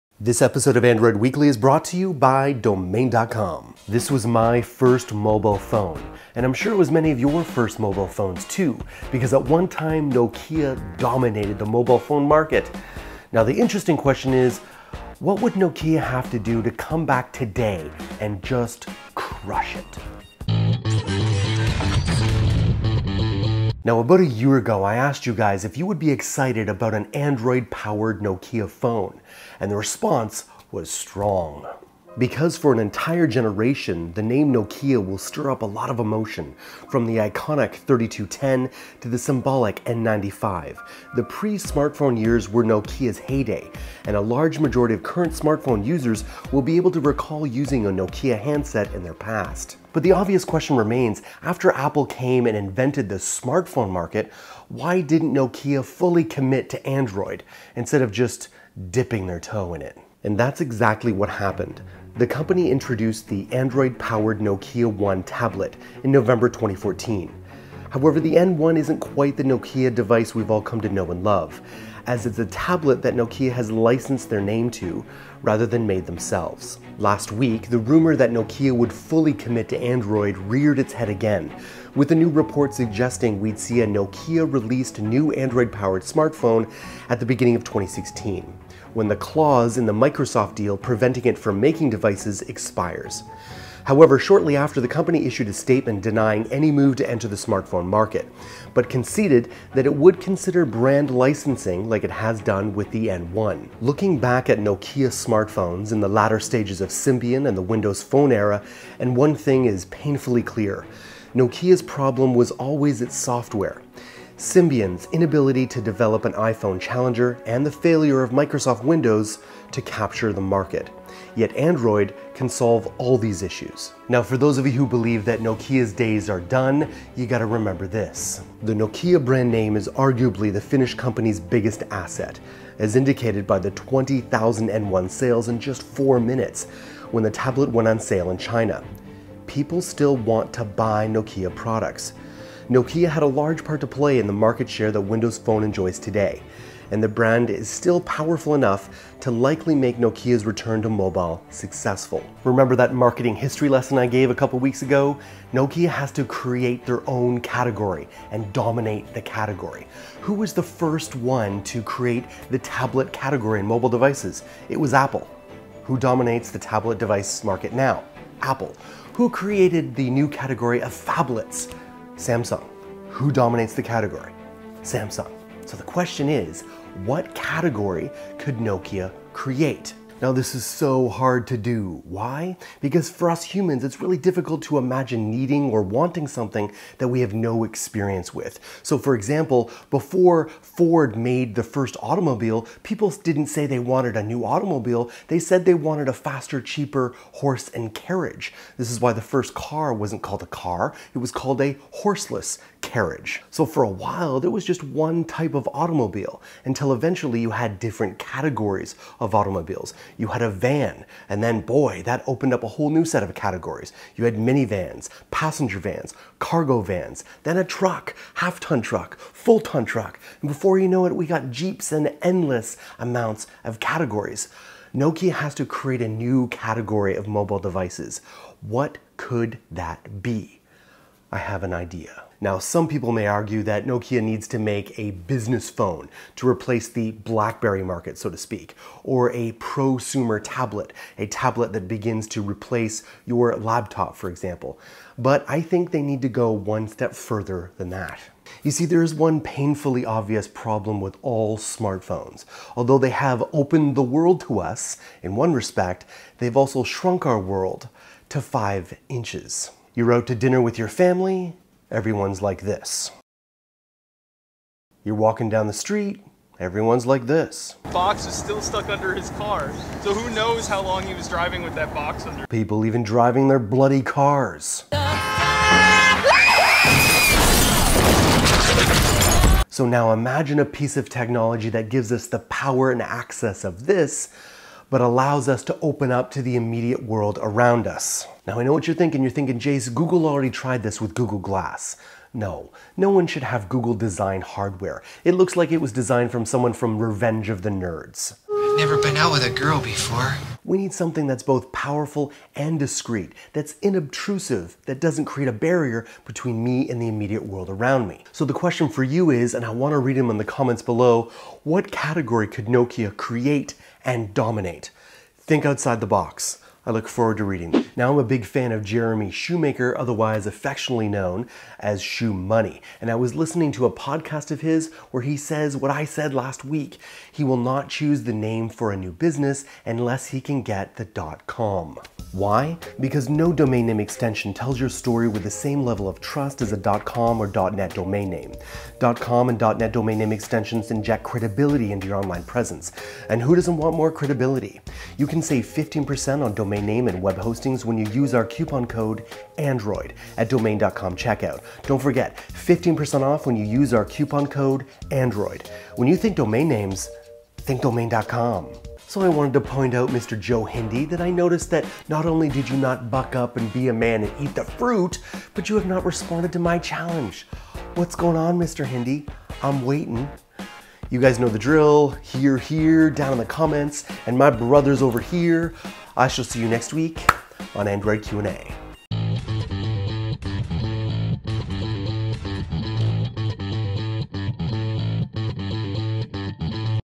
How Nokia Could Return To Dominance - Android Q&A
checkout

Jayce talks about what Nokia might do to return to dominate the mobile device world